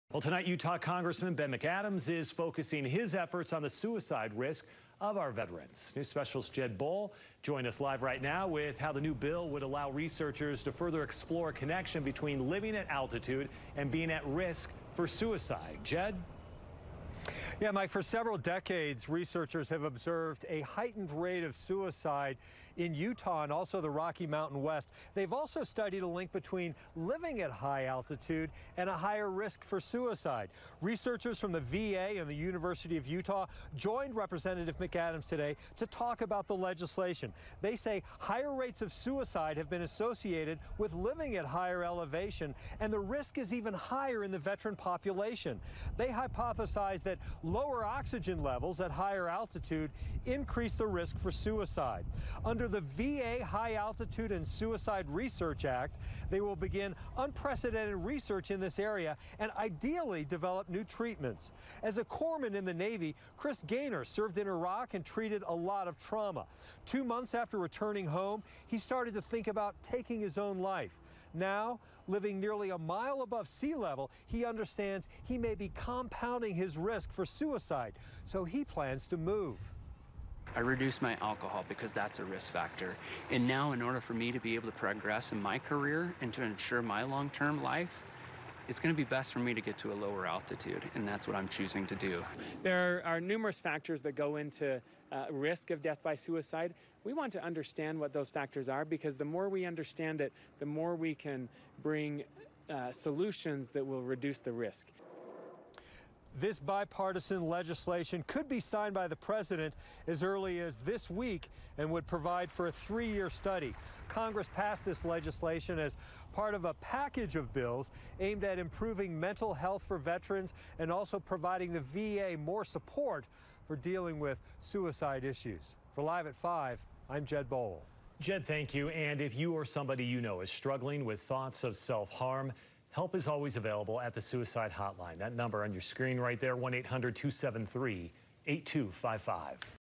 Rep. McAdams Bill Exploring Link Between Elevation, Veteran Suicides
Utah Congressman Ben McAdams has focused his efforts on the suicide risk of Utah's veterans, sponsoring a new bill that would allow researchers to further explore a connection between living in high altitudes and being at-risk for suicide.